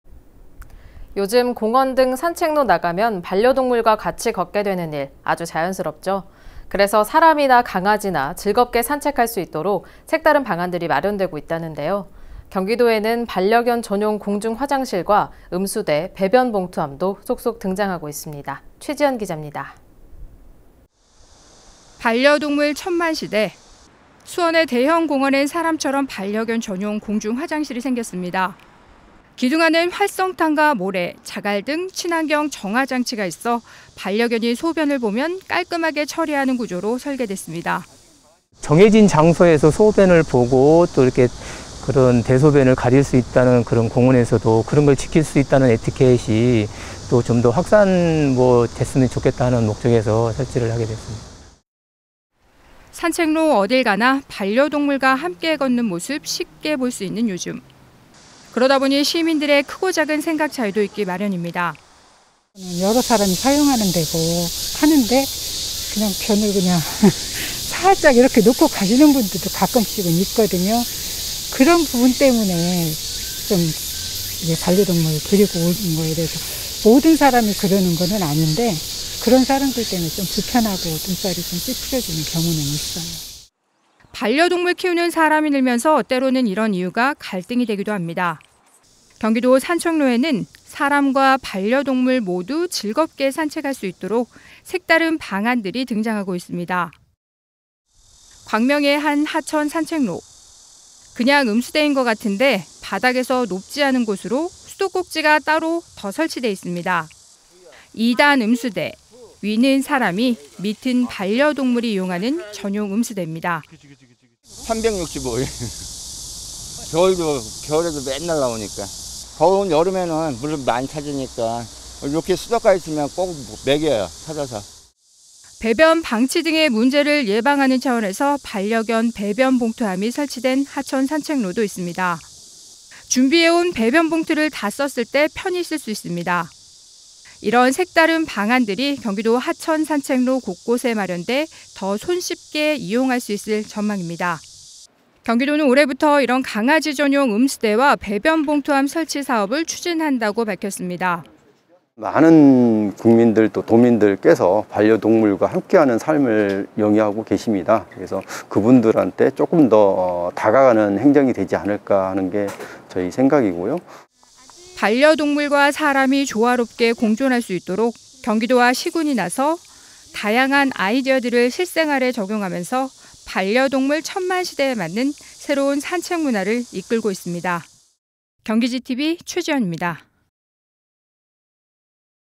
반려동물 천만..전용 화장실‧음수대까지
[앵커멘트]
요즘 공원 등 산책로 나가면 반려동물과 같이 걷게 되는 일, 아주 자연스럽죠. 그래서 사람이나 강아지나 즐겁게 산책 잘 할 수 있도록 색다른 방안들이 마련되고 있다는데요. 경기도에는 반려견 전용 공중 화장실과 음수대, 배변봉투함도 속속 등장하고 있습니다. 최지현 기자입니다.

[리포트]
반려동물 천만 시대.
수원의 대형 공원에 사람처럼 반려견 전용 공중화장실이 생겼습니다.
기둥 안엔 활성탄과 모래, 자갈 등 친환경 정화정치가 있어 반려견이 소변을 보면 깔끔하게 처리하는 구조로 설계 됐습니다.
[인터뷰]박선봉/수원시 공원녹지사업소 호수공원팀장
정해진 장소에서 소변을 보고 대소변을 가릴 수 있다는, 공원에서도 그런 것을 지킬 수 있다는 반려견 에티켓이 좀 더 확산됐으면 좋겠다는 목적에서 설치를 하게 됐습니다.

(화면전환)산책로 어딜 가나 반려동물과 함께 걷는 모습 쉽게 볼 수 있는 요즘.
그러다 보니 시민들의 크고 작은 생각 차이도 있기 마련입니다.
[인터뷰]이춘옥/성남시 정자동
여러 사람이 사용하는 곳이고 한데 대변을 살짝 이렇게 놓고 가시는 분들도
가끔씩은 있거든요. 그런 부분 때문에, 반려동물 데리고 오거나 하시면 모든 사람들이 그런 건 아닌데 그런 분들 있으시면 좀 불편하고 눈살이 찌푸려지는 경우는 좀 있어요.

반려동물 키우는 사람이 늘면서, 때로는 이런 이유가 갈등이 되기도 합니다.
경기도 산책로에는 사람과 반려동물 모두 즐겁게 산책 할 수 있도록 색다른 방안들이 등장하고 있습니다.

광명의 한 하천 산책로.
그냥 음수대인거 같은데 바닥에서 높지 않은 곳으로 수도꼭지가 따로 더 설치 돼 있습니다.
2단 음수대.
위는 사람이, 밑은 반려동물이 이용하는 전용 음수대입니다.
[인터뷰]조덕형/광명시 광명5동
365일, 겨울에도 맨날 산책 나와요. 더운 여름에는 물을 많이 찾으니까 이렇게 수돗가 있으면 꼭 먹여요. 찾아서….

배변 방치 등의 문제를 예방하는 차원에서 반려견 배변 봉투함이 설치된 하천산책로도 있습니다. 준비해 온 배변봉투를 다 썼을 때 편히 쓸 수 있습니다.
이런 색다른 방안들이 경기도 하천 산책로 곳곳에 마련 돼 더 손쉽게 이용할 수 있을 전망입니다.
경기도는 올해부터 이런 강아지 전용 음수대와 배변봉투함 설치 사업을 추진한다고 밝혔습니다.
[인터뷰]이명선/경기도 하천정책팀장
많은 국민들, 도민들께서 반려동물과 함께하는 삶을 영위하고 계십니다. 그래서 그분들한테 조금 더 다가가는 행정이 되지 않을까 하는 게 저희 생각입니다.

반려동물과 사람이 조화롭게 공존할 수 있도록 경기도와 시군이 나서 다양한 아이디어들을 실생활에 적용하면서 반려동물 천만 시대에 맞는 새로운 산책문화를 이끌고 있습니다.

영상취재: 나인선, 영상편집: 김상환

[자막]

1. 수원 광교호수공원
2. 반려견 전용 공중화장실 눈길
3. [인터뷰]박선봉/수원시 공원녹지사업소 호수공원팀장
4. [인터뷰]이춘옥/성남시 정자동
5.광명 목감천 산책로
6. [인터뷰]조덕형/광명시 광명5동
7.성남 탄천 산책로
8. 경기도, 하천 산책로 반려견 음수대 및 배변 봉투함 설치 시범사업 추진
9. [인터뷰]이명선/경기도 하천정책팀장